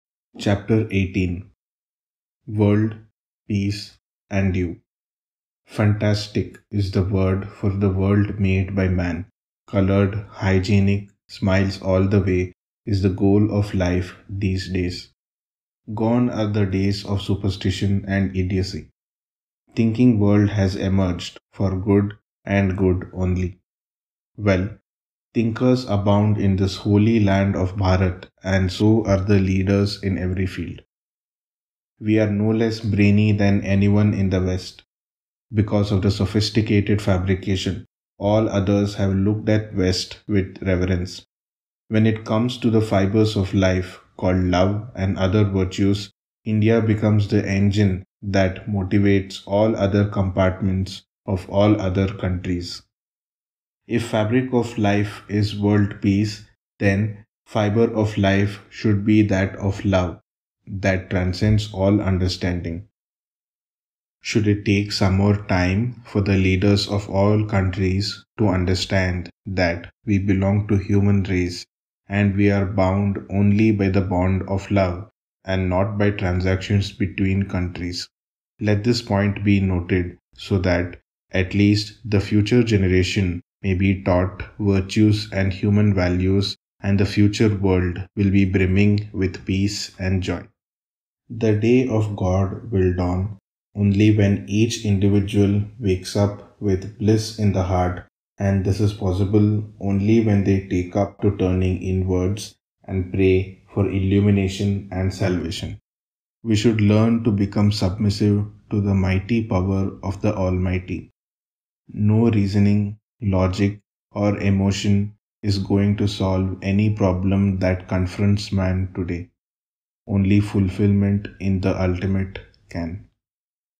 The Book PERFECTION - Chapter 18 - WORLD, PEACE AND YOU
The Book - 'Perfection' - The key to your FREEDOM.
A collection of discourses by Guruji Viswanath, the Greatest Mystic.

You may not reproduce this in any way without the express written permission of Manava Seva Kendra

Voice Over - Sashank Chittipeddi